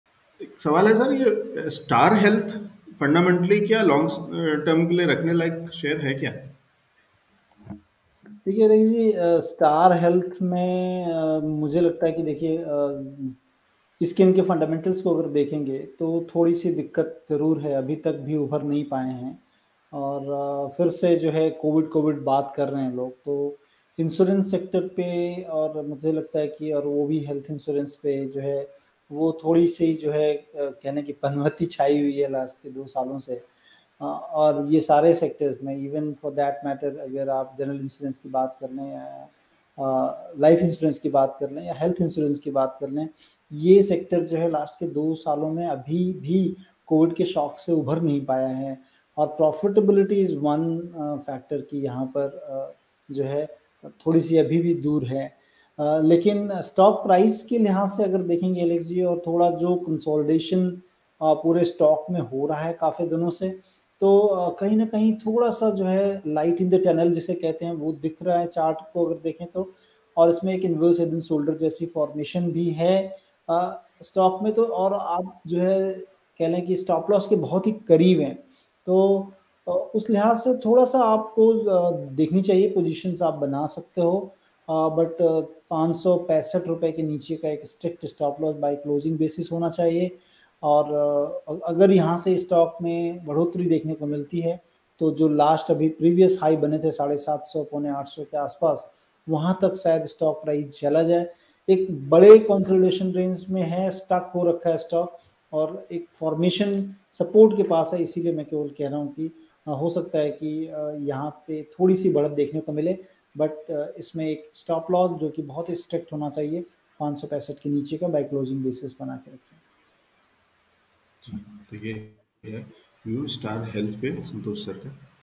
Star Health and Allied Insrnce Com Ltd. के शेयर में क्या करें? Expert Opinion by Santosh Singh
जानिए Stock Market Expert Santosh Singh की राय |

Analysis of: Star Health and Allied Insrnce Com Ltd.

Shows on starhealth starhealthandalliesinsurance StockMarket equity trading investment commodity personalfinance reviews marketanalysis startups nifty sensex and much more.